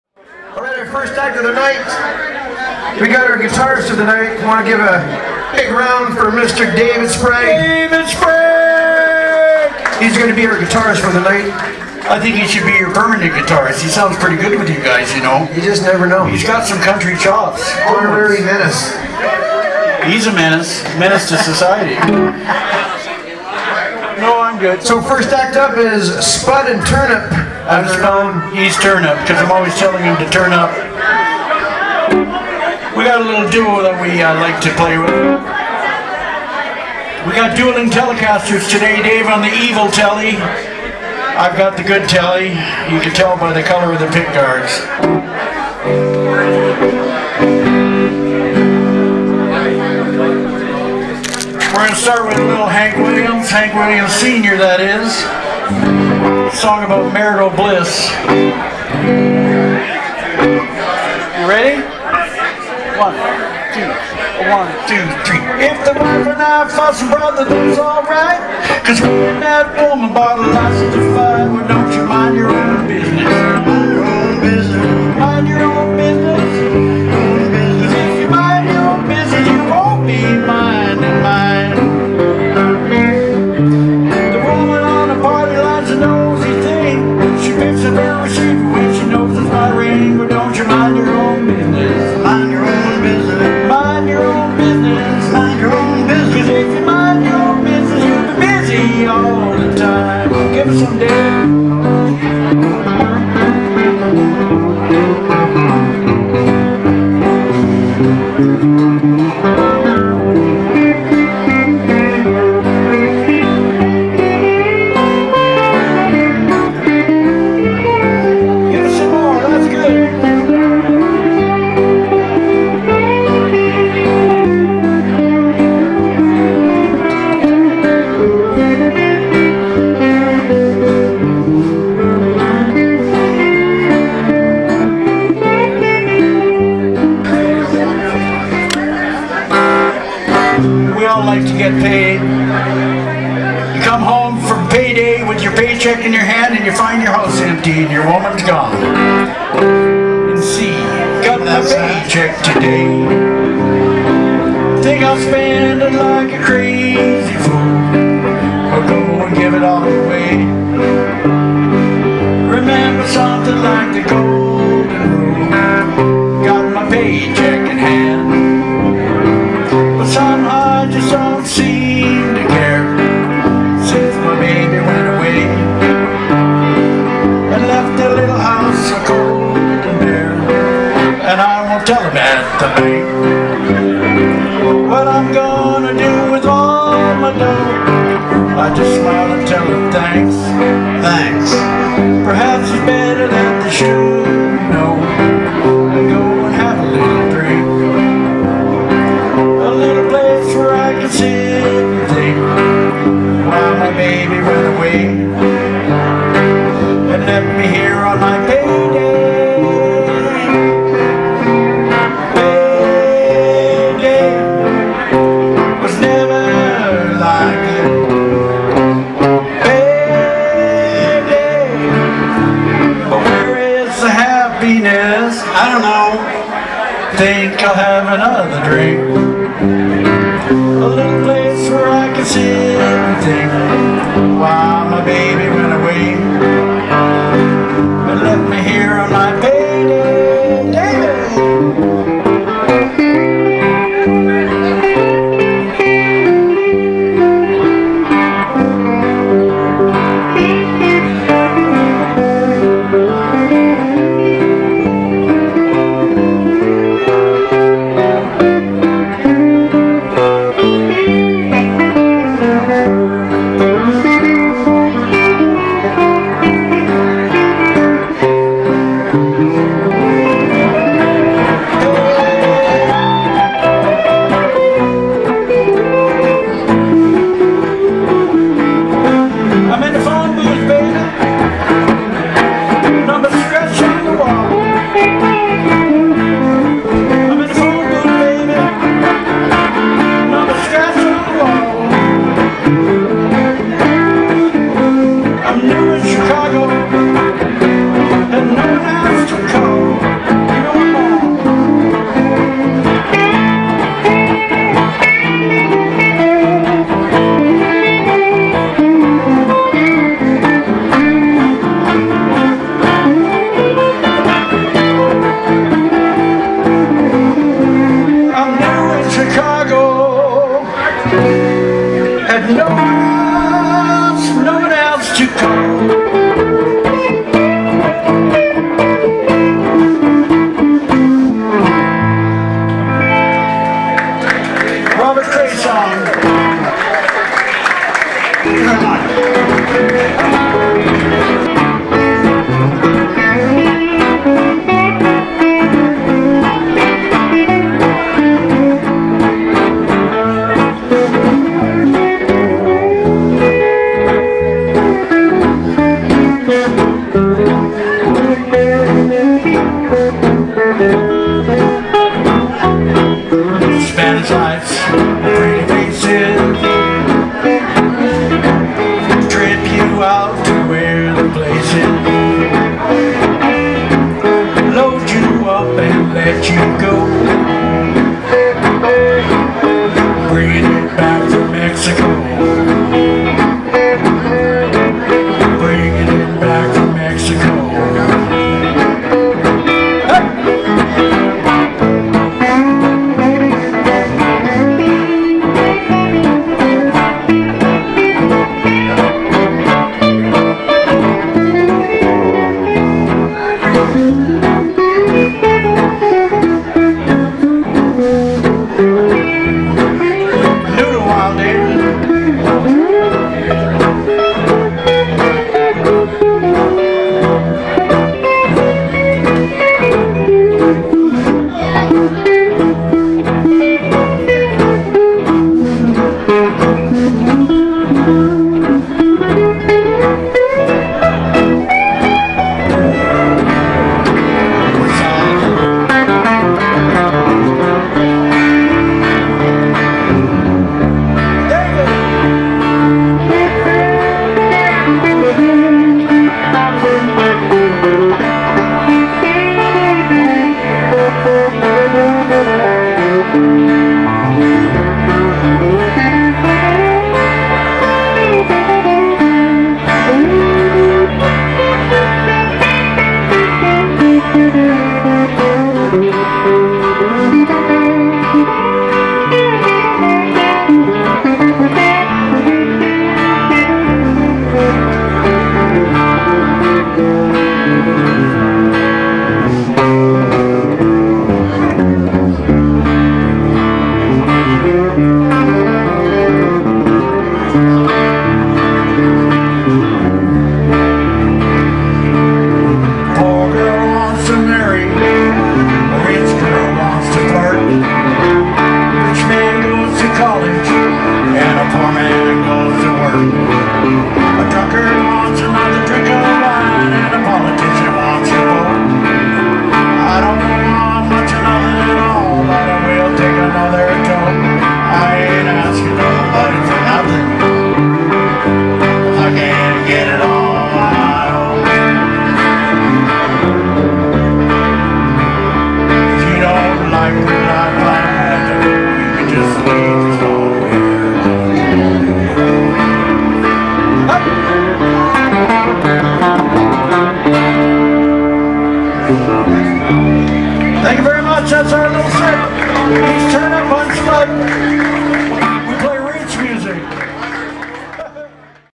Spud and Turnip play roots music some sampler clips
Spud and Turnip play roots music some sampler clips from the Carlson Club jam.